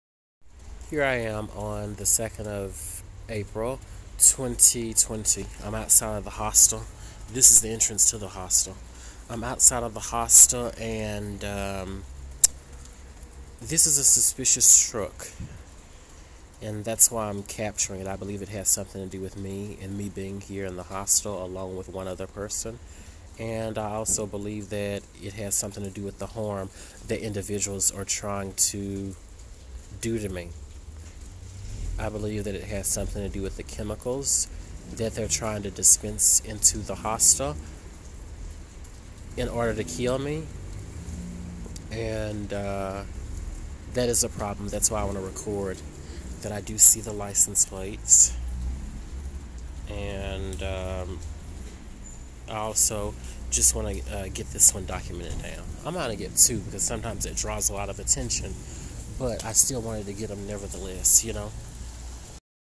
American Targeted Individual: Hostel Inbox (Macedonia, Skopje)
This is the truck that I was lucky and fortunate enough to catch outside of the hostel where I am staying. After I started to prepare documents for my book, this is when this electrical truck appeared. The gap between when I started to solidify content for my book and now has been less than 48 hours. The intent of the white supremacists from the American Government working covertly behind the Macedonian Police and also the owners of Inbox Hostel and Hotel De Koka.